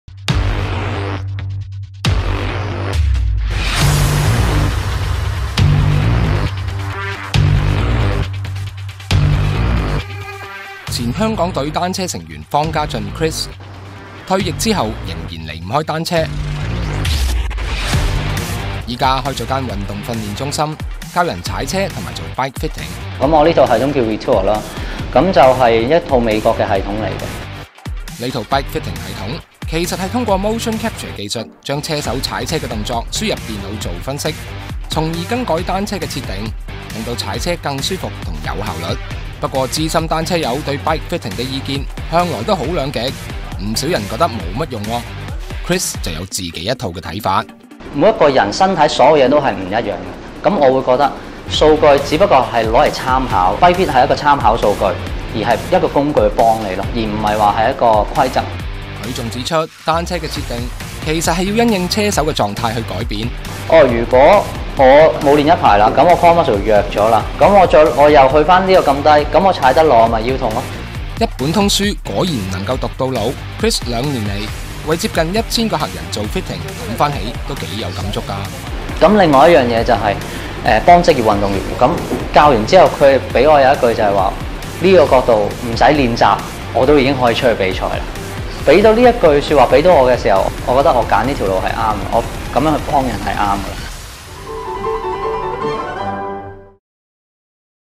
單車設定師 調整有道踩得省力
「踩多啲啦，踩多啲冇事㗎喇！」前香港單車代表隊成員方家進（Christopher）回想當年練習遇上痛楚時，教練說過的話。那時候仍未流行單車設定（Bike Fitting），座墊是高是低、把手是遠是近，一切靠估，然後專業運動員的練習量遇上錯誤姿勢，傷痛便隨之而來。為了使其他人不必走自己的舊路，方家進退役後便遠赴美國學習，為每位車手度身，調整單車設定。

《果籽》 栽種品味，一籽了然。